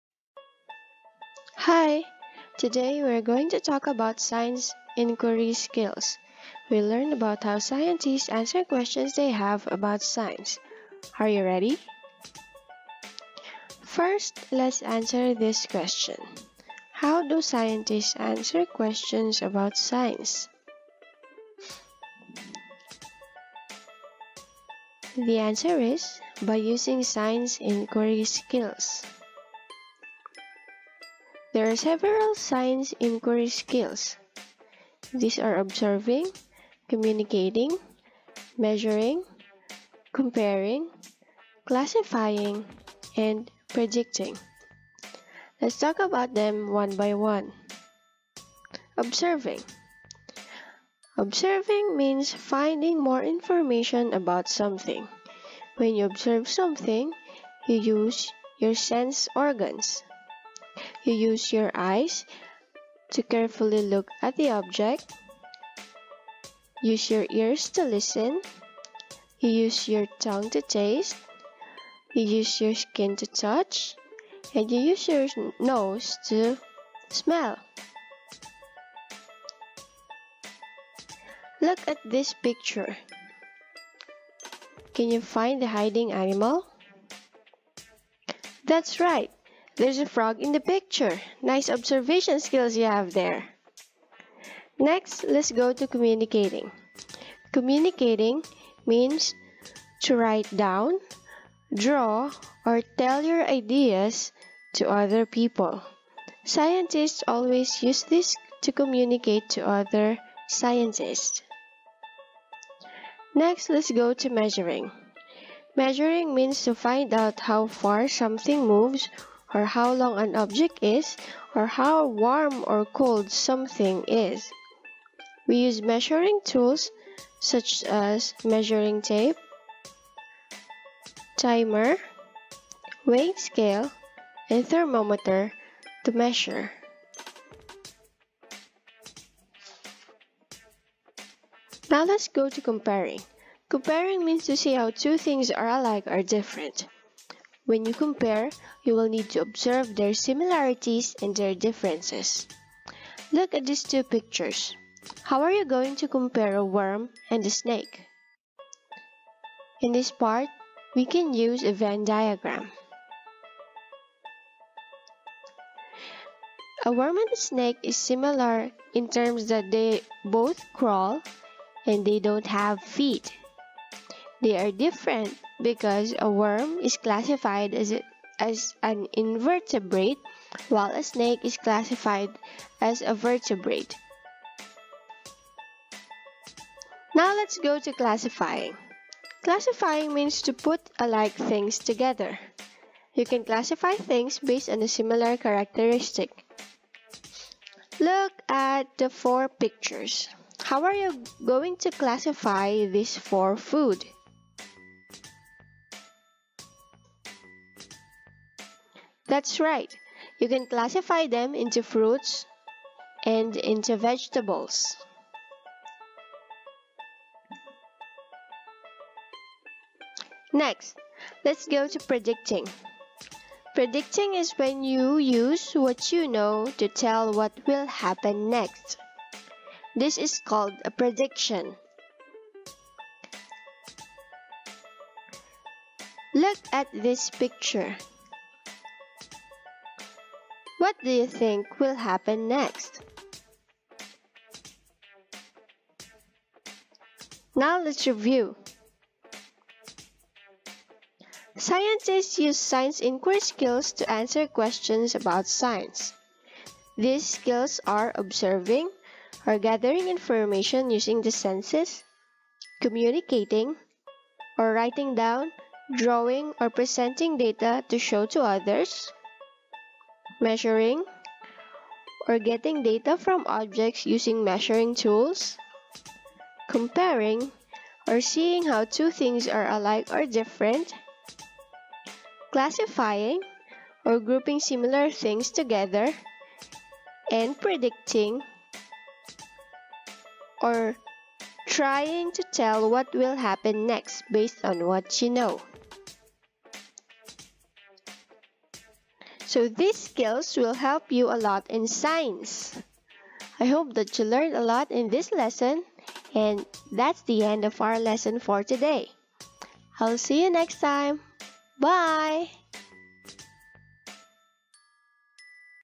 Science Inquiry Skills Grade 3 Science
How do Scientists answer questions about Sciece?

Lets learn Science Inquiry Skills - Observing, Communicating, Measuring, Comparing, Classifying and Predicting